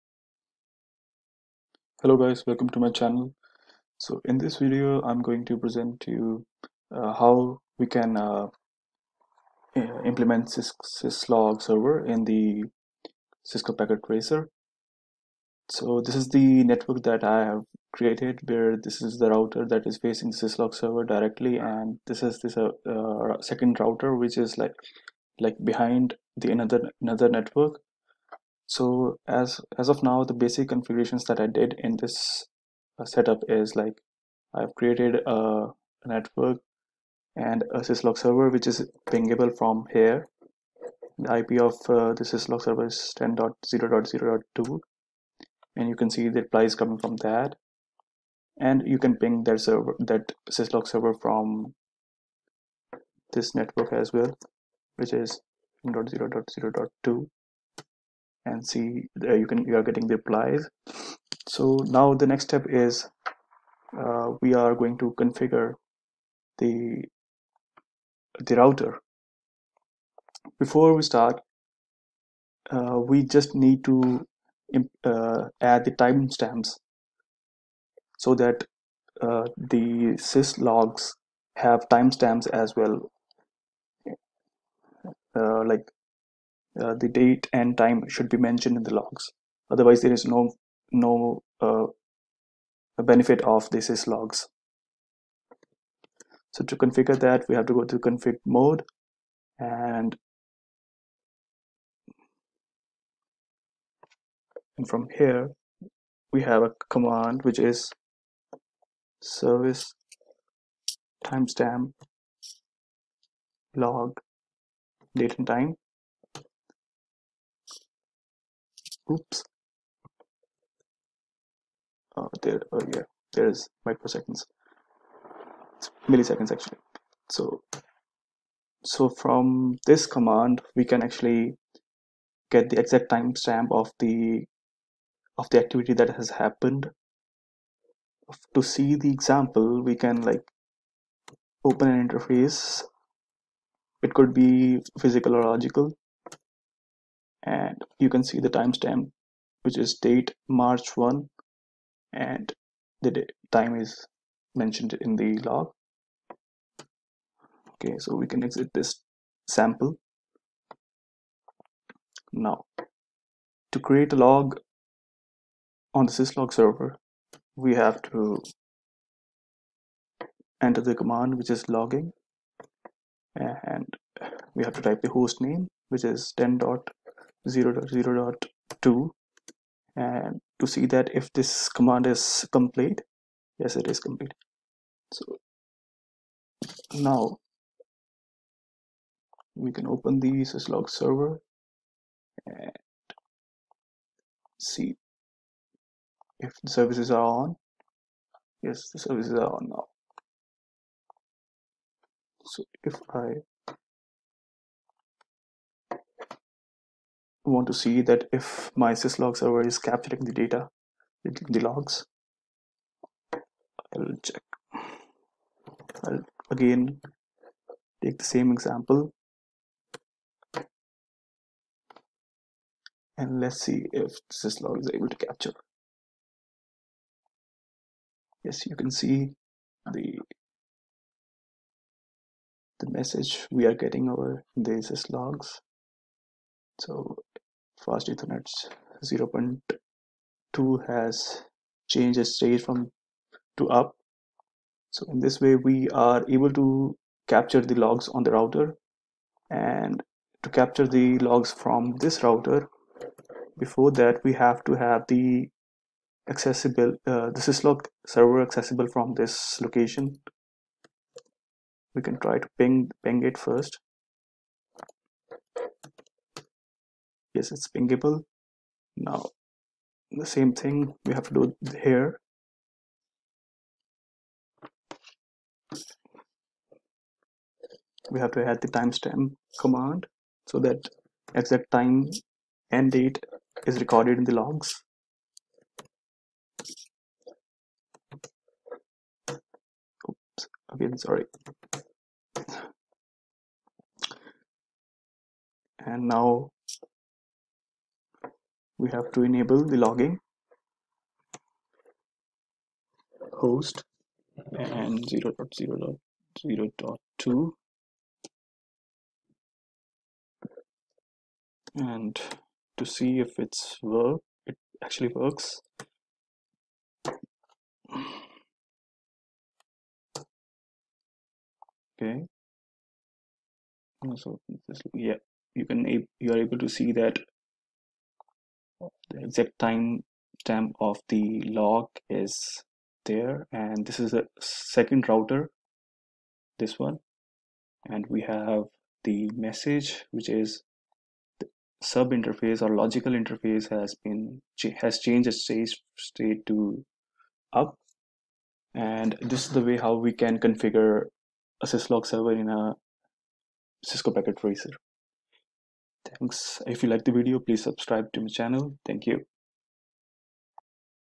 Syslog server | Cisco packet tracer
This is one of the many steps to ensure the network is secure. Logging in a central Syslog server helps us to maintain logs in an efficient way.
Steps are mentioned below:
1. Assign IP to the interfaces of both the routers.
2. Configure routing protocols like RIP, OSPF, or a static route.
3. Check the reachability of the Syslog server.
3. Add timestamps to the logs on routers.
4. Enable logging on the routers.
5. Start Syslog service on the server.